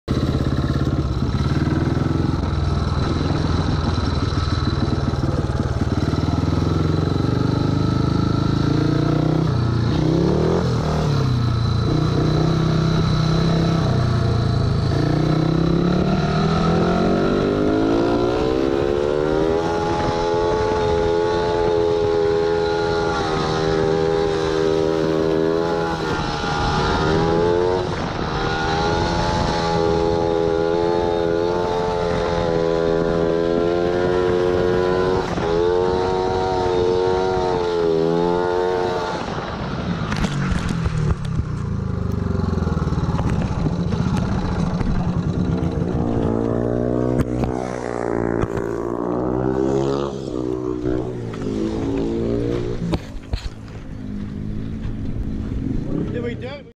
Jedd List's First Ride On A 500cc Speedway Bike
Despite the laydown, 15-year-old 250cc world number three, Jedd List was really impressive on his first ride on a 500.